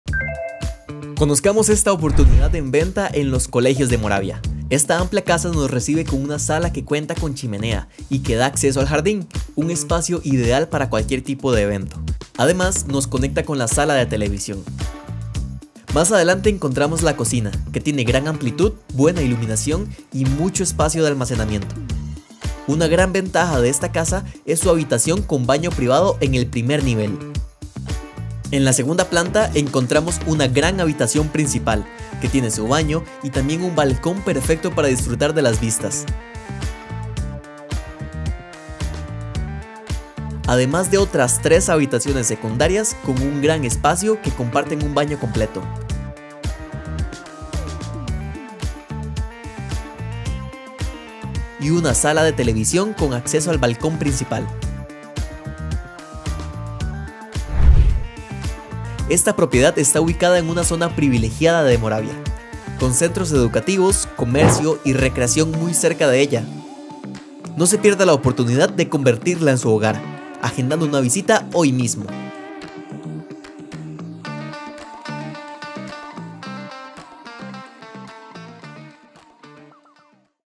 Venta de Amplia Casa en Los Colegios de Moravia 🏡✨ 36965
🏡 🔑 🌳  Enlace directo a la propiedad: 🛋️ 🛁 💼

Esta hermosa casa de dos niveles le ofrece todo lo que necesita para vivir cómodamente. En la primera planta, encontrará un jardín verde, estacionamiento para dos vehículos bajo techo y espacio para dos más sin techar. Al ingresar, un recibidor le da la bienvenida, seguido de un medio baño para visitas y una habitación de buen tamaño con baño completo y acceso con rampa ♿️.

La cocina separada cuenta con muebles de melamina y sobres de granito, ideal para los amantes de la cocina 🍽️. El comedor con chimenea y la sala separada crean un ambiente acogedor para disfrutar en familia 🔥. Además, hay una bodega, terraza, patio con áreas en cerámica y verde, y un área de pilas.

En el segundo piso, encontrará un espacio para TV, un gran balcón hacia la calle, tres habitaciones muy amplias, todas con closet de pared y que comparten un baño completo. El dormitorio principal tiene su propio balcón y baño privado, ofreciendo un espacio de tranquilidad y confort 🌟.

La casa cuenta con agua caliente, portón eléctrico vehicular y peatonal, cerca electrificada al frente y alarma, garantizando su seguridad 🔐.

¡No pierda esta oportunidad de vivir en una zona privilegiada con todas las comodidades! Agende una visita hoy mismo y haga de esta casa su nuevo hogar 🏡💫.

📞 ¿Prefiere atención personal? Llame al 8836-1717 o al WhatsApp 📲